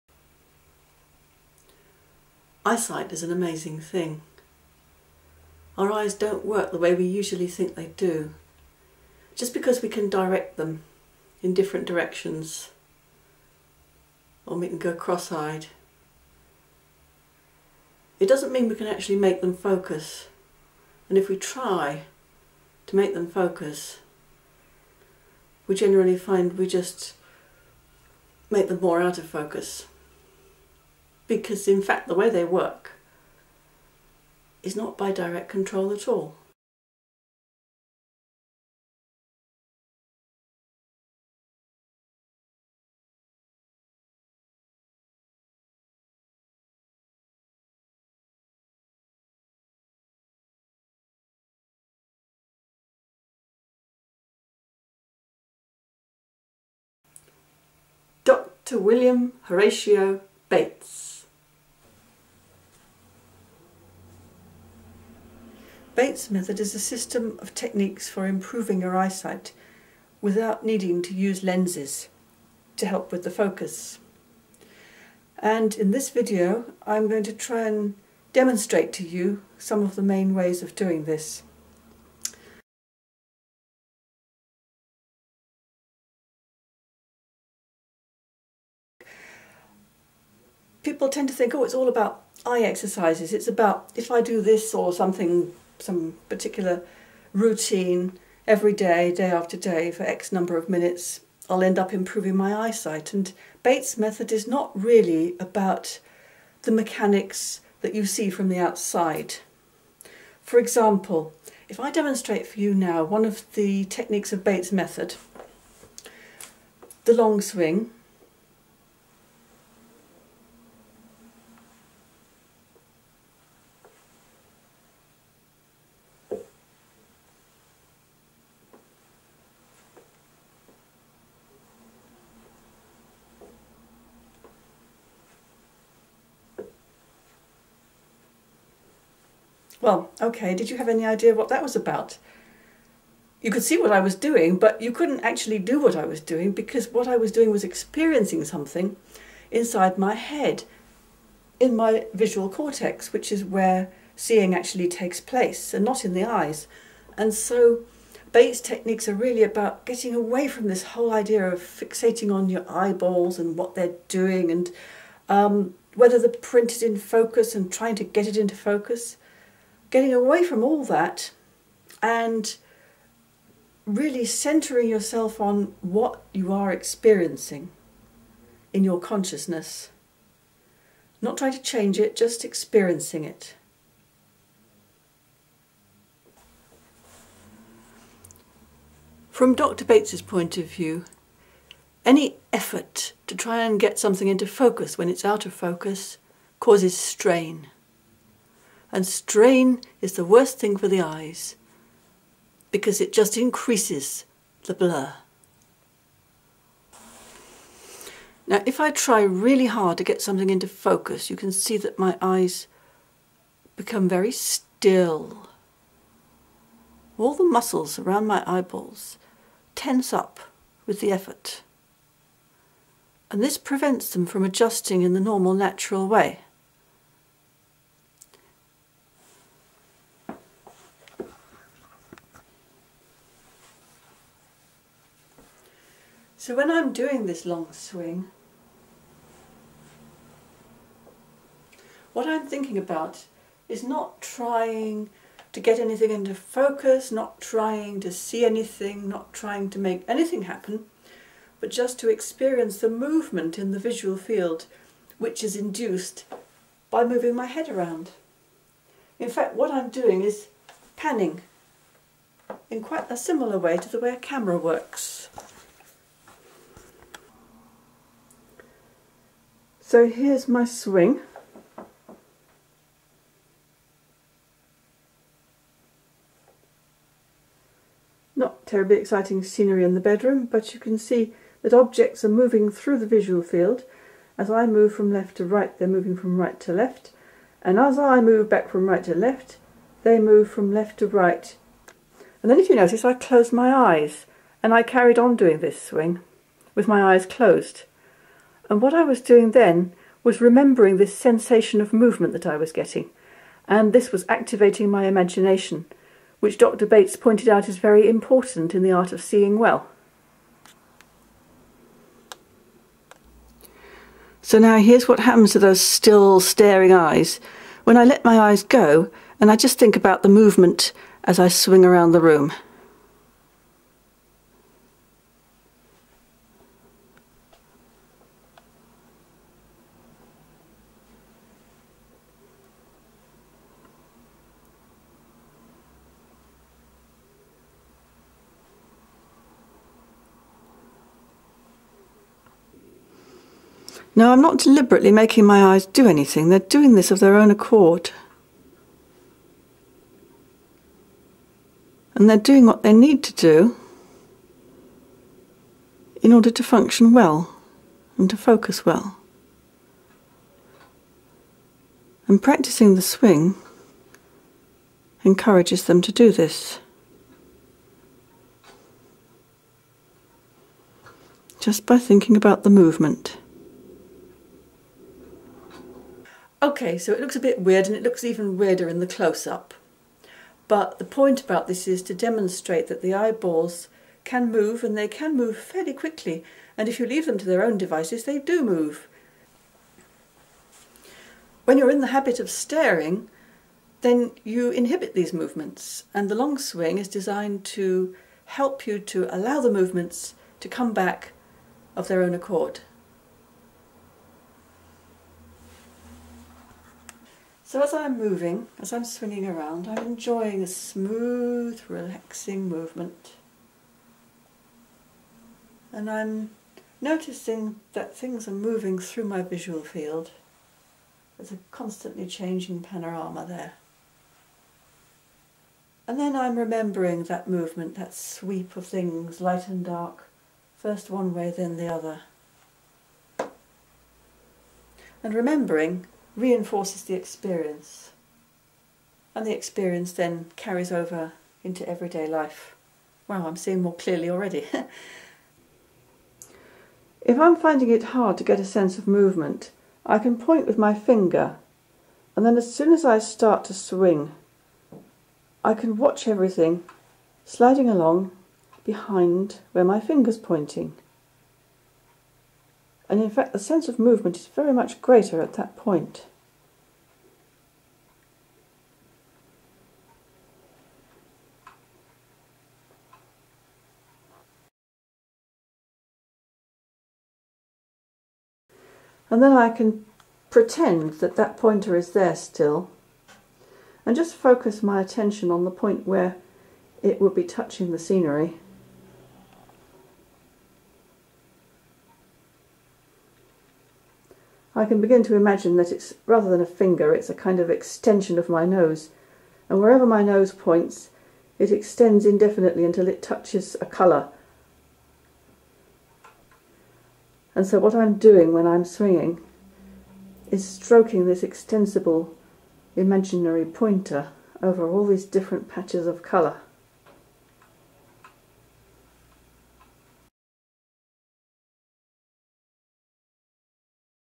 The Bates Method for natural eyesight improvement
Introducing the concepts of Bates method for natural vision improvement.

How to let your eyes move freely instead of fixing and staring, which leads to strain. Seeing takes place in the brain, using the eyes as sensors. To improve focus you have to let your eyes go, and focus instead on the experience of visual movement that you are having inside your head.

When you relax and stop trying to control your eyes, they find a better focus naturally through the stimulation of the movement that you are noticing.